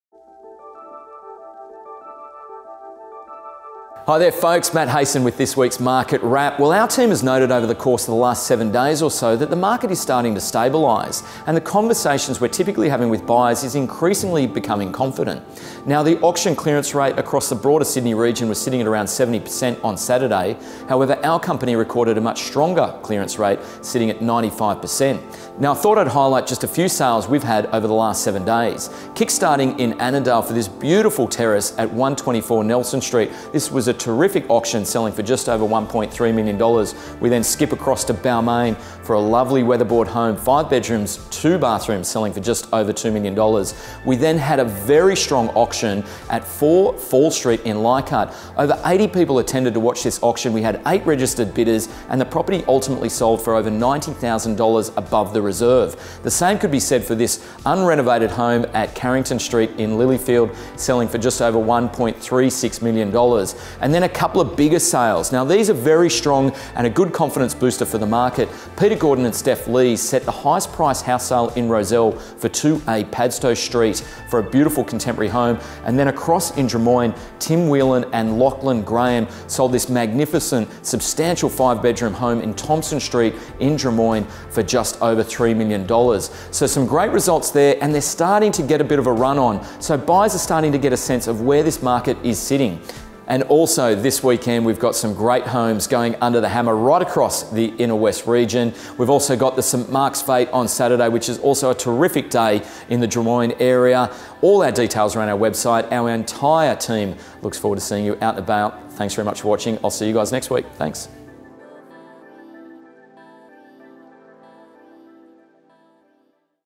Weekly Market Pulse - 10 March 2016 - with Matthew Hayson
Whether you’re looking to buy, sell or just keeping an eye on the market, you’ll want to watch Matt Hayson’s weekly Market Pulse to hear our take on the past week’s market activity including:

- Is the local market finding its feet?
- We take a look at some results over the past week that suggest it is.
- Two records set for 2016 in the prestige home sector

We have a wonderful selection of properties open this weekend and don’t miss St Mark’s Fete on Saturday.